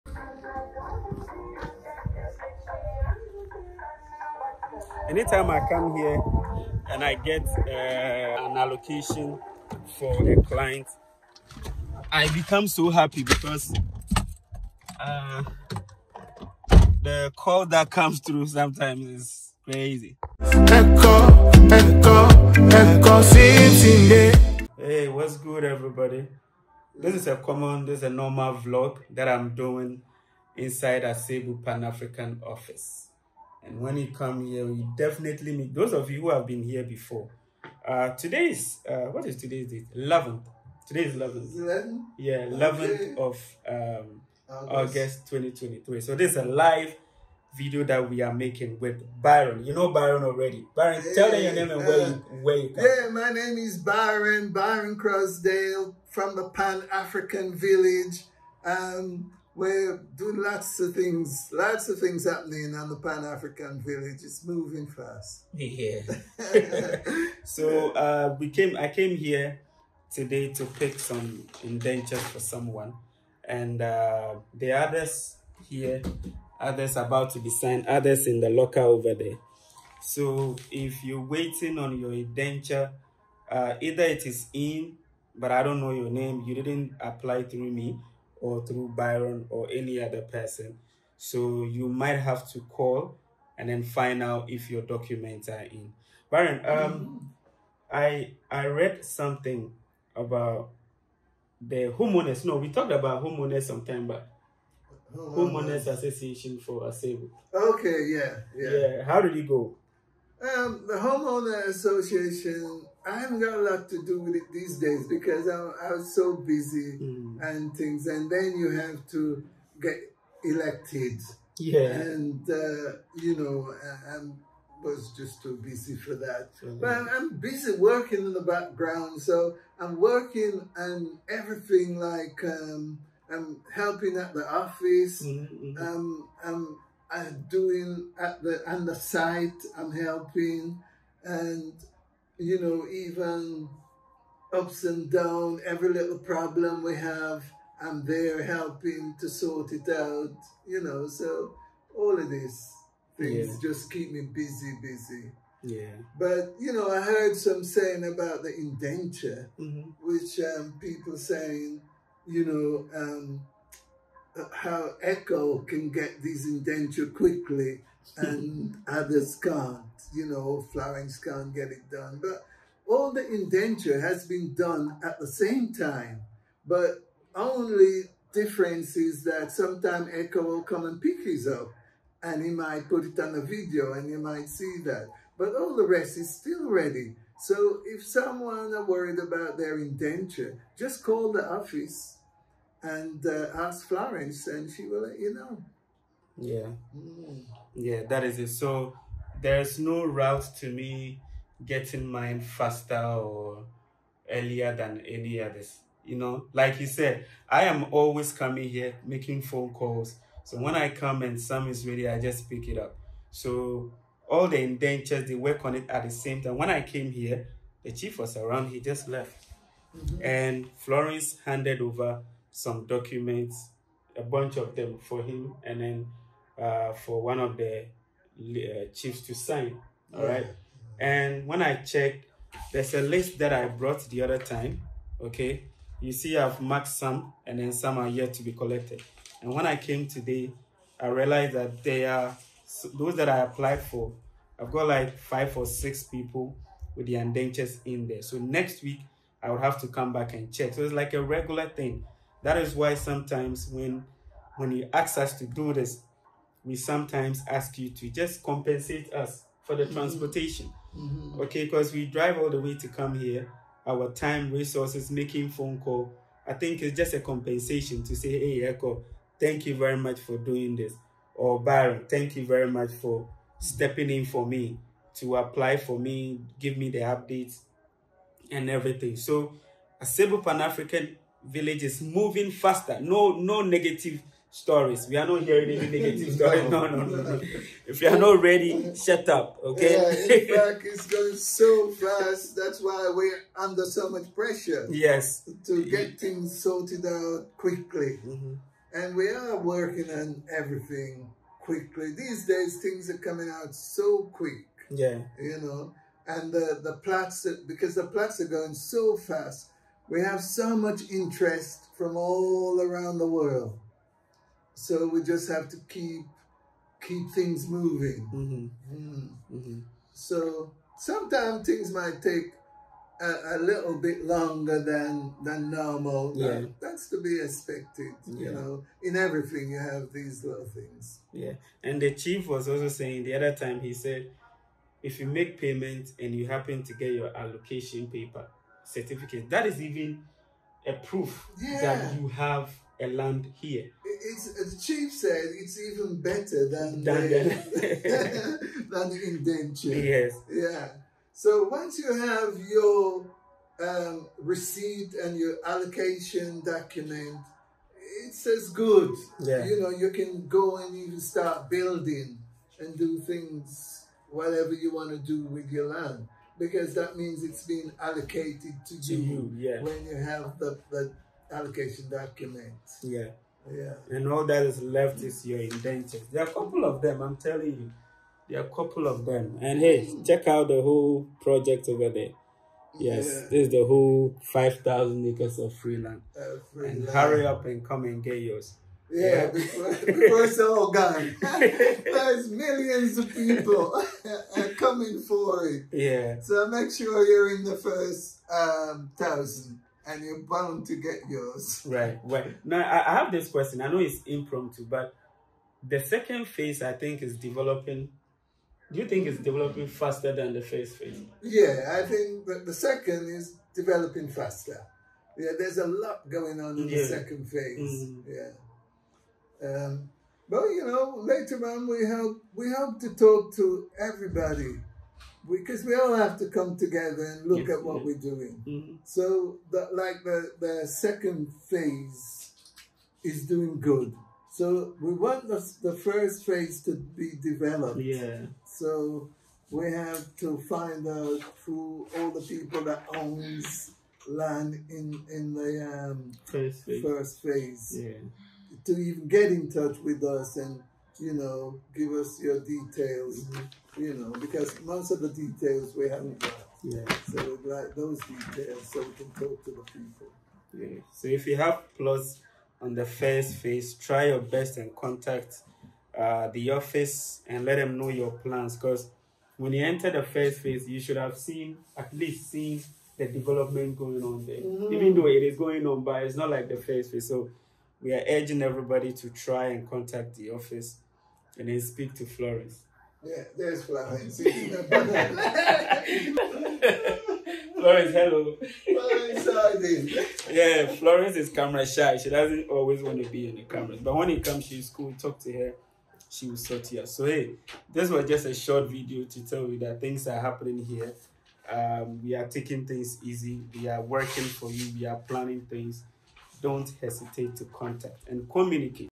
Diaspora Resident In Asebu Talks About The 5000 Acres Of Free Land Offered To All Diasporas
A Diaspora Resident in Asebu Talks About The 5000 Free Land Offered To All Diasporas.